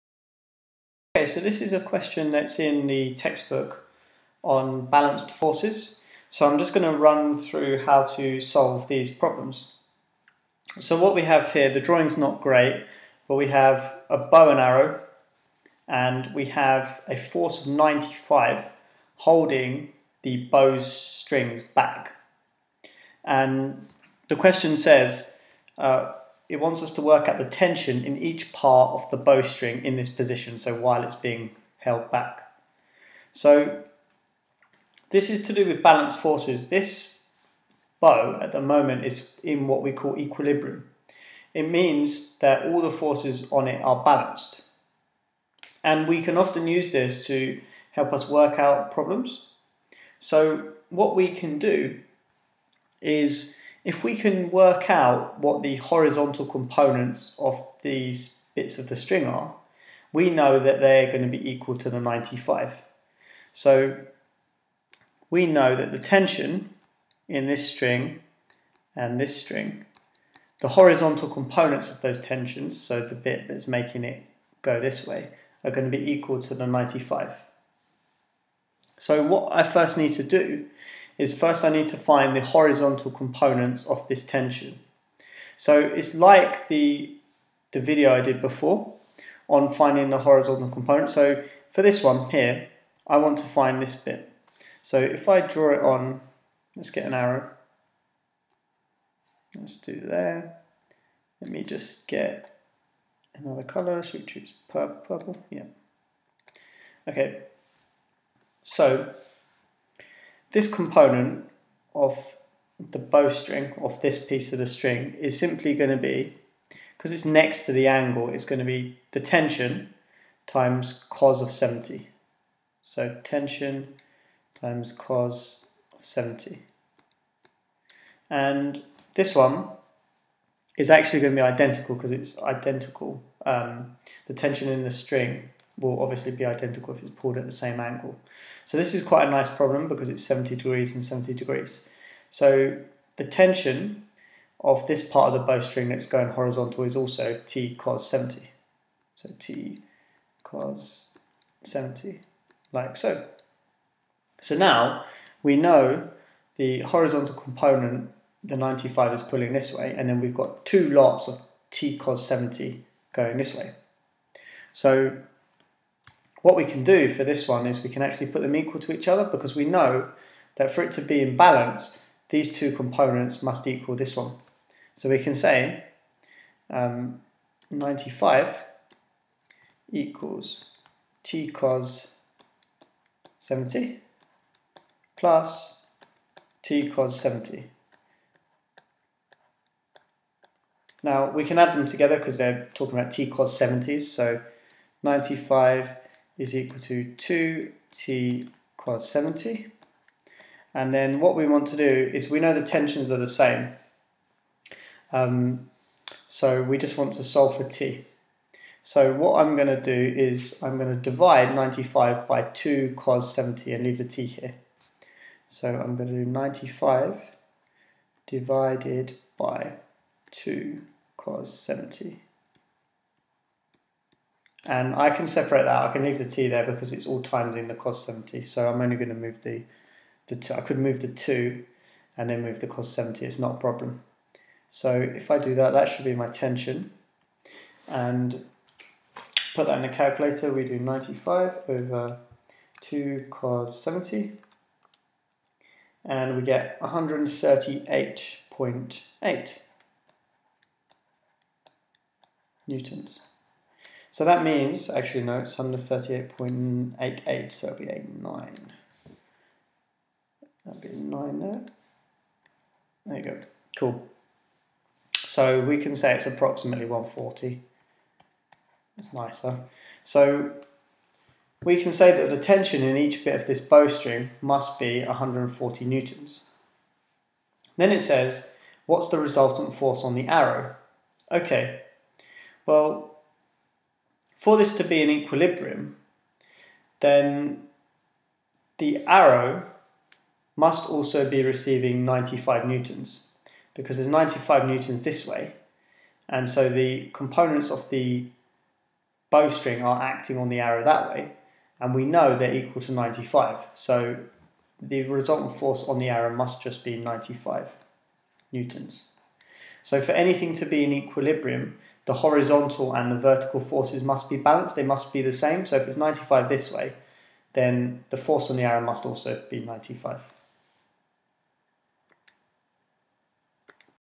AQA A-Level-Balanced Forces, Bowstring Question From Textbook
How to solve the bowstring problem from the textbook.